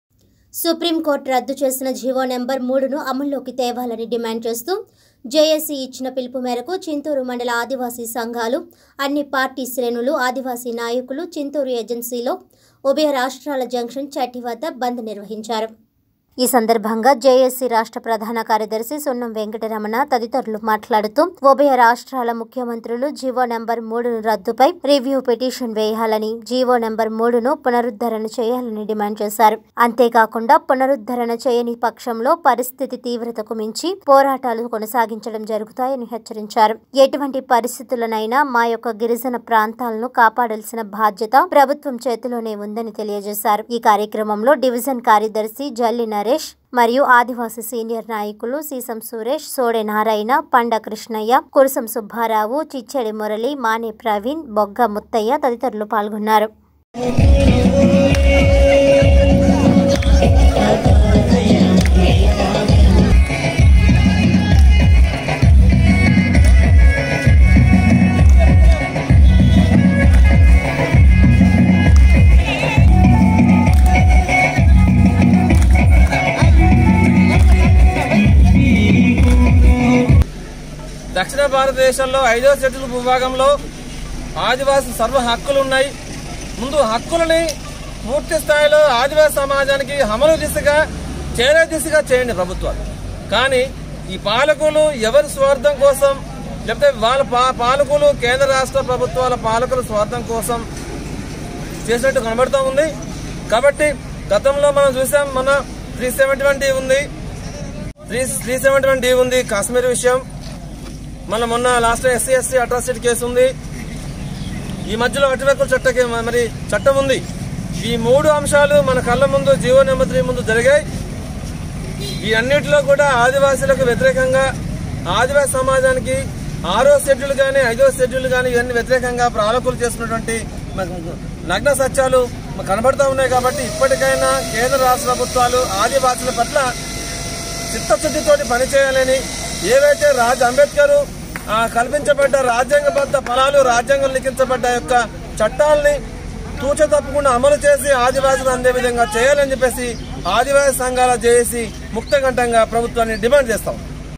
చింతూరు - జి.ఓ నెంబర్ 3 ను అమలులోకి తేవాలని ఆదివాసీ సంఘాలు డిమాడ్ _18_June_2020_TV 7 News_E.G.Dt
సుప్రీం కోర్టు  రద్దు చేసిన జి.ఓ నెంబర్ 3 ను అమలులోకి తేవాలని డిమాడ్ చేస్తూ జే.ఏ.సి ఇచ్చిన పిలుపు మేరకు చింతూరు మండల ఆదివాసీ సంఘాలు, అన్ని పార్టీ శ్రేణుల ఆదివాసీ నాయకులు, చింతూరు ఏజెనిలో ఉభయరాష్ట్రాల జంక్షన్ చట్టి వద్ద బంద్ నిర్వహించారు .